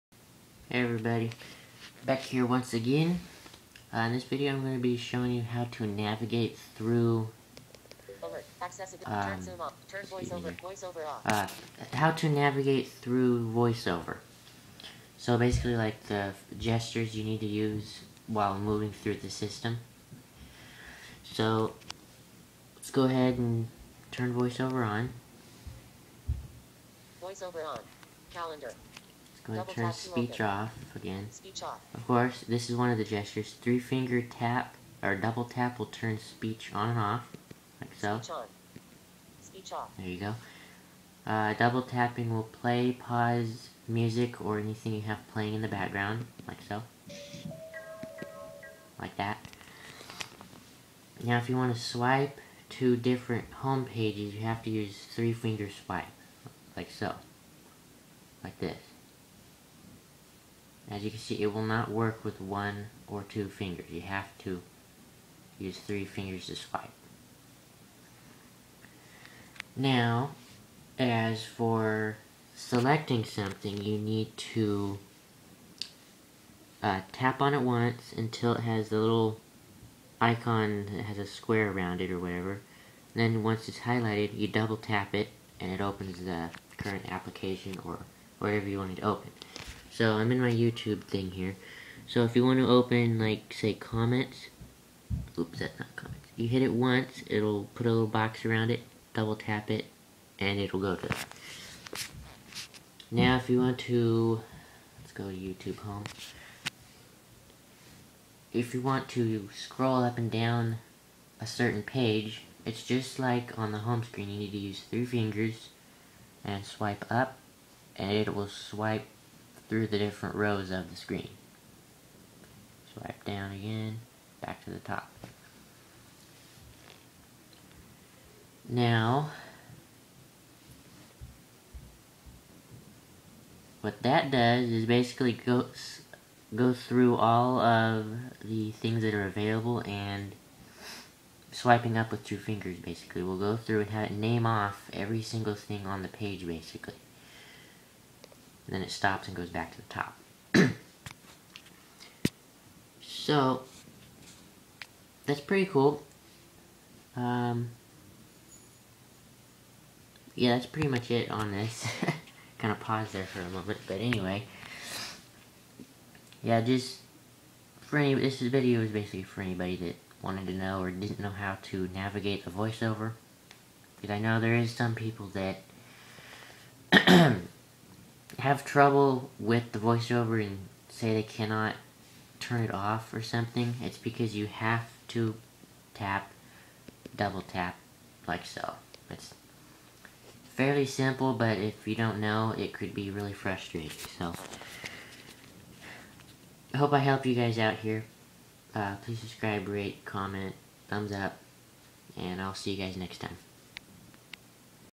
How to navigate through iOS with VoiceOver on
These are the main gestures you need to know when using VoiceOver.